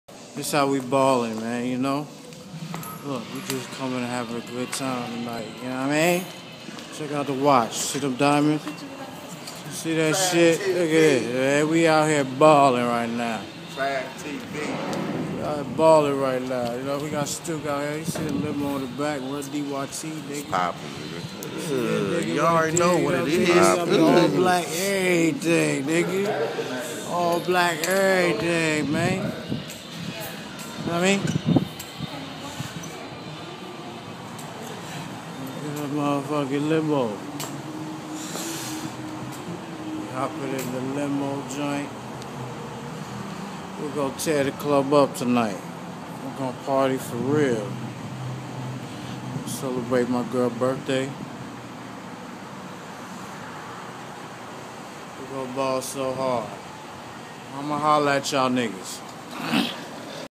Traffik. On the way to the club
St. Petersburg Florida   / Traffik with my family kicking it 4 bleak a birthday